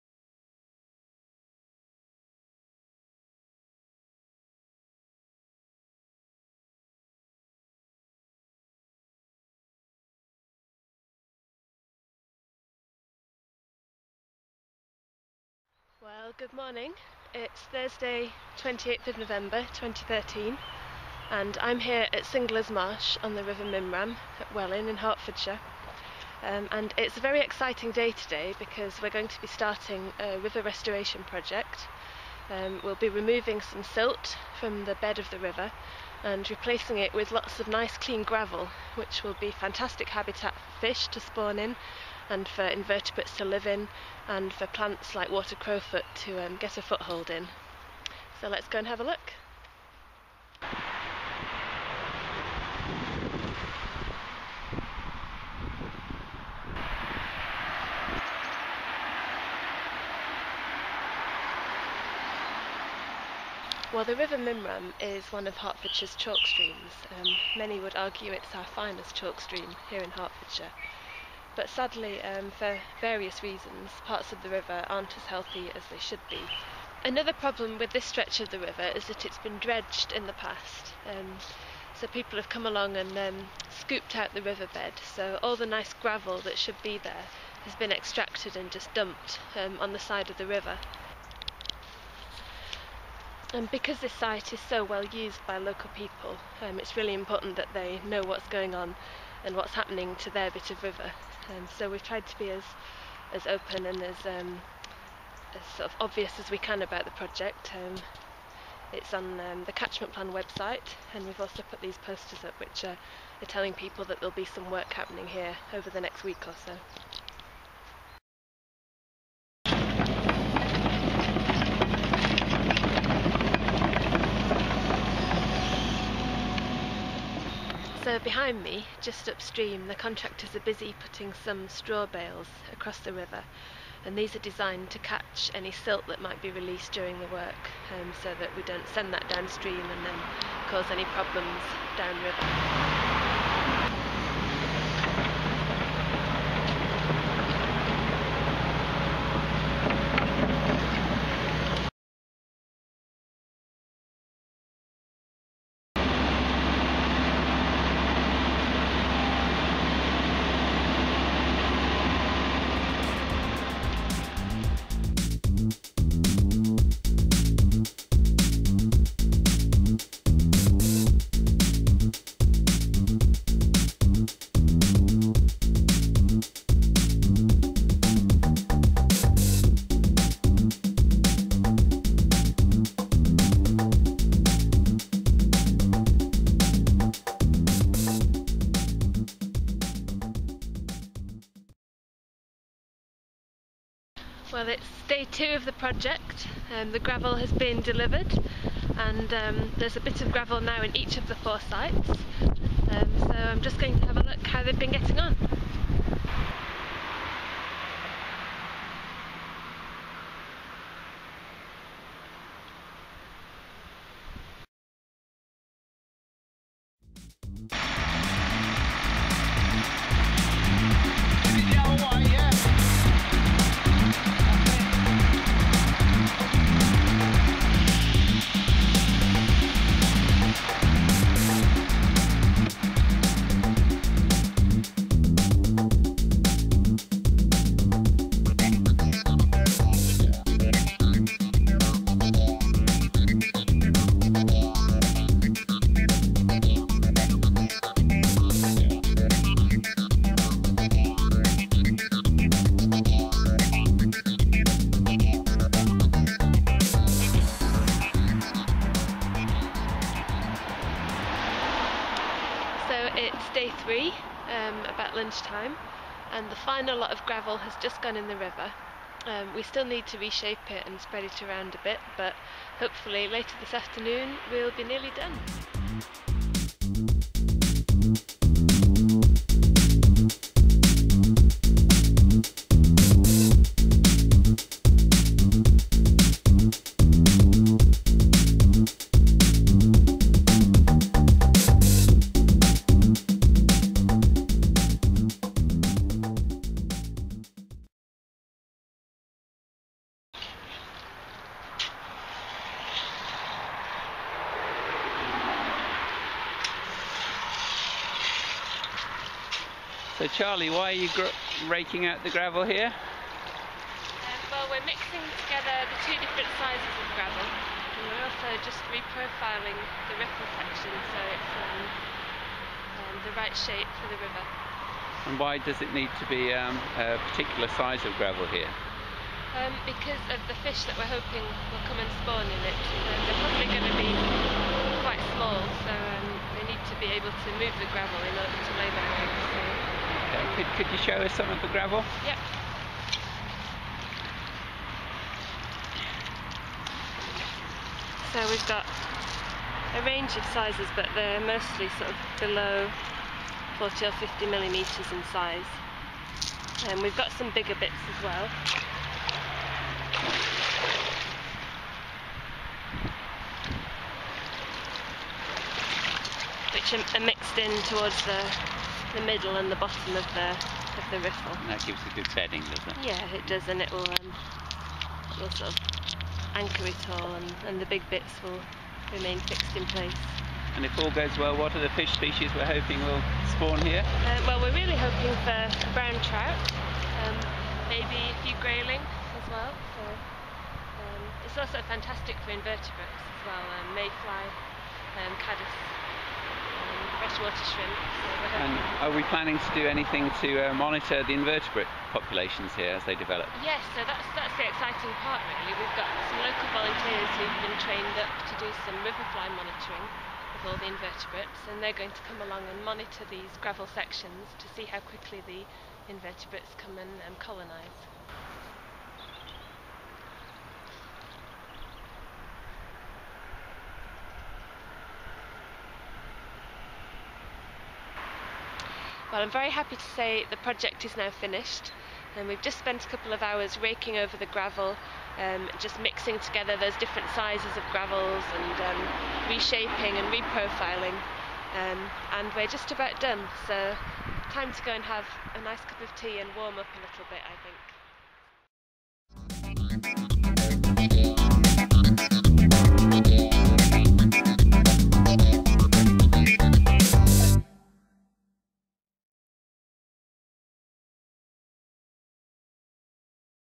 River restoration at Singler's Marsh
Hertfordshire Living Rivers Officer Charlie Bell takes you through HMWT's recent project to recreate areas of gravel riffle on the River Mimram at Singler's Marsh, Welwyn, Hertfordshire.  60 tonnes of silt was removed from the river bed and replaced with 60 tonnes of gravel!